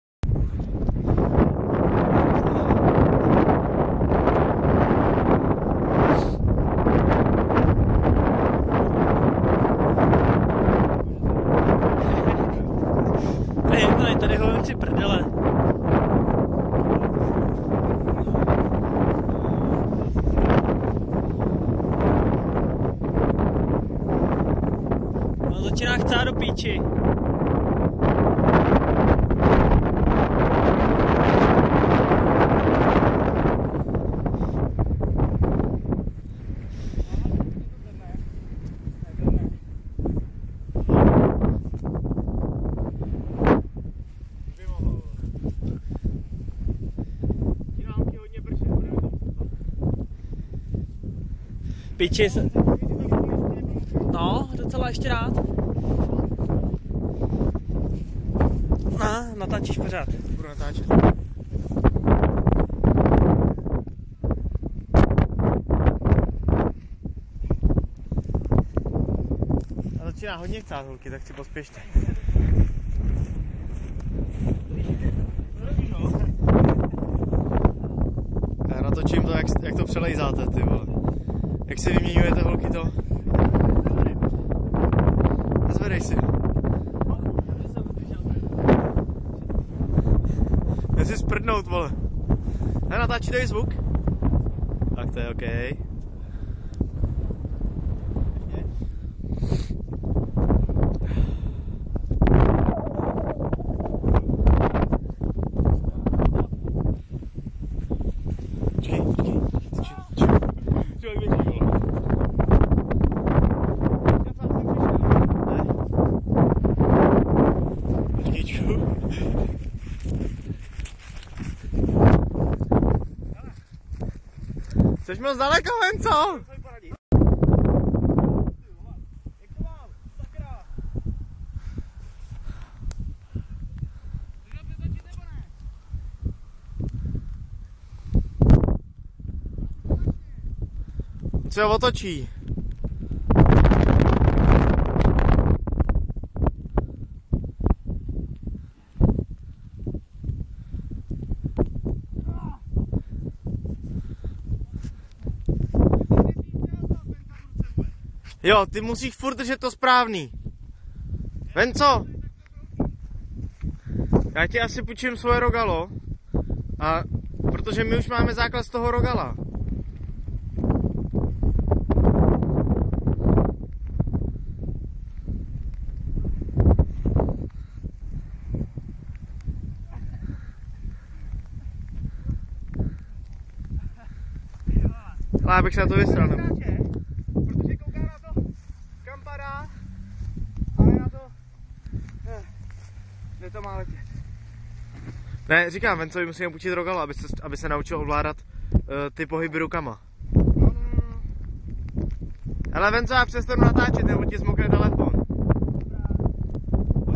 Blbci z Chrastavy 2, Kite Pansh 4.5m
Další díl z dílny třech trotlů z Chrastavy, který maj rádi dráčky